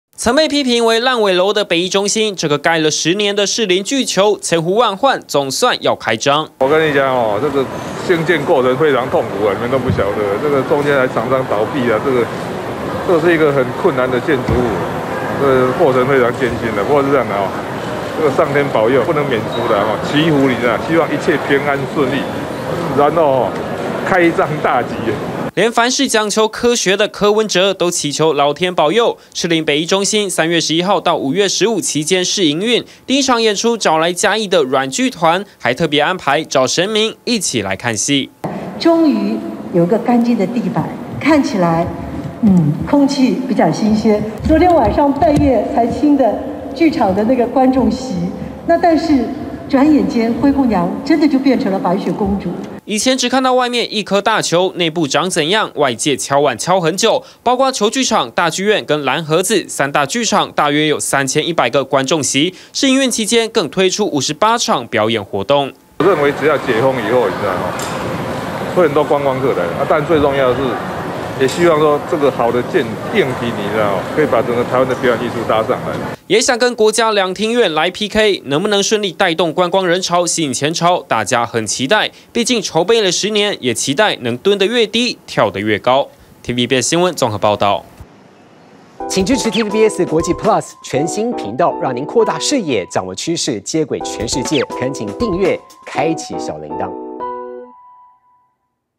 士林巨球「北藝中心」試營運 柯文哲:上天保佑｜TVBS新聞
大家到士林夜市附近，最有印象的就是一棟有顆巨大球體的建築物，而它就是台北表演藝術中心，前前後後蓋了10年時間，如今總算要開張試營運了，時間從3/11一直到5/15，而第一場演出還特別安排，找神明一起來看戲，台北市長 柯文哲也說，真的希望上天保佑，試營運能夠順利。